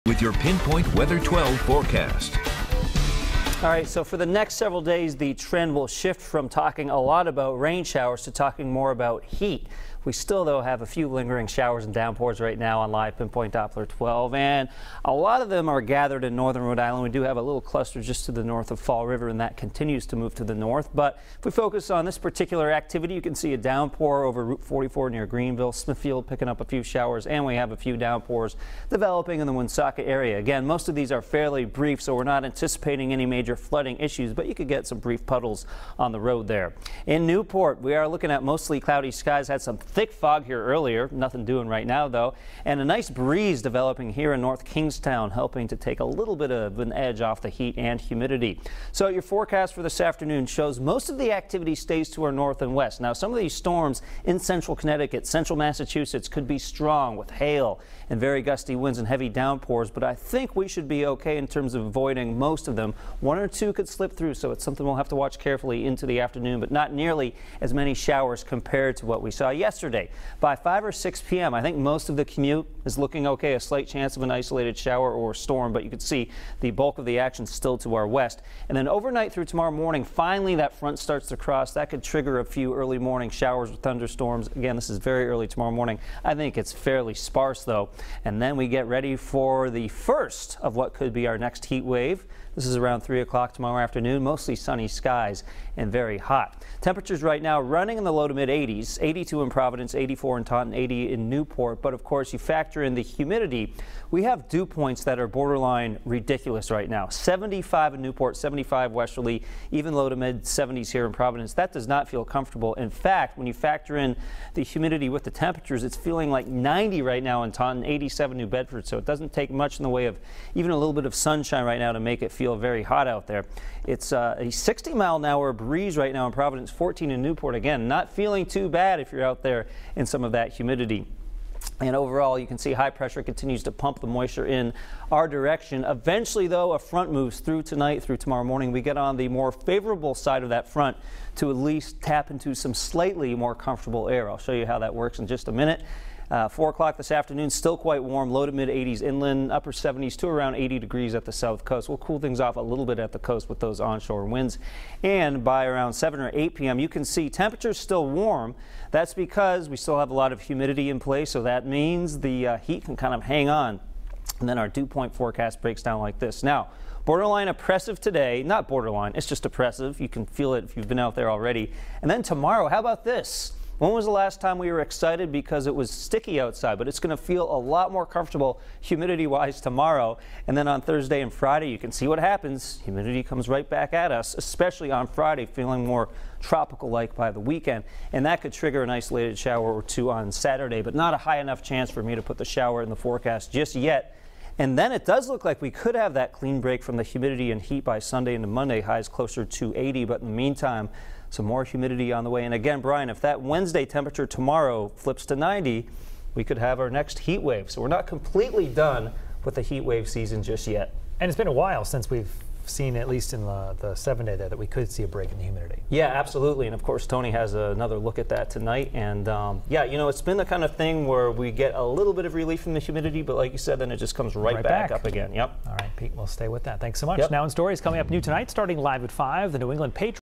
Pinpoint Weather 12 Forecast
Meteorologist Pete Mangione has your forecast for Tuesday August 14, 2018.